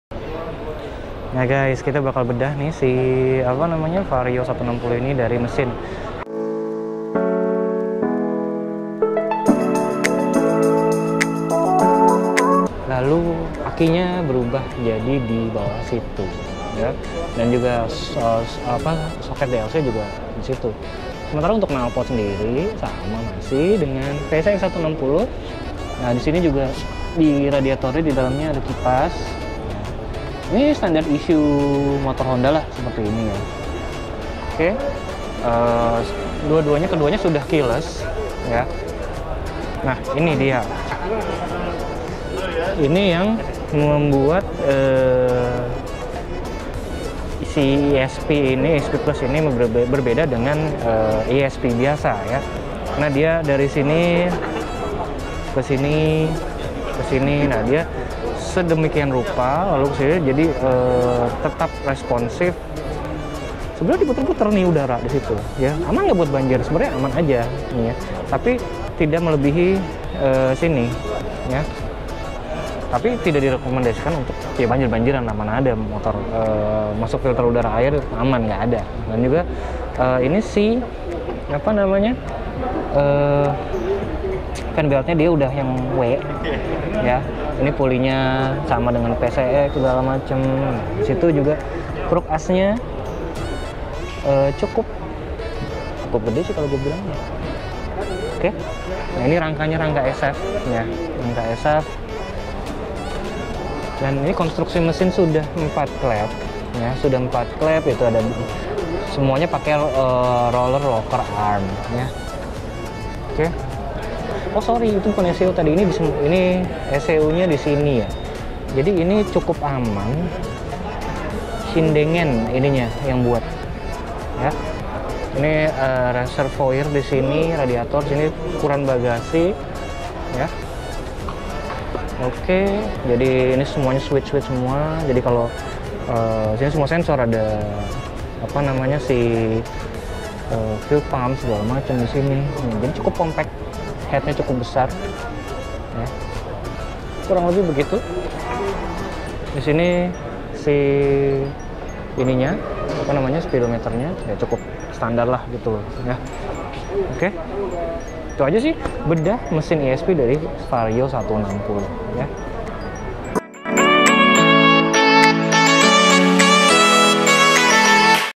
HONDA VARIO 160, BEDAH MESIN DAN TEKNOLOGI (2023)
Hari ini kita akan bedah mesin dan teknologi honda vario 160 Esp+, kayak gimana? langsung ke video aja ya guys..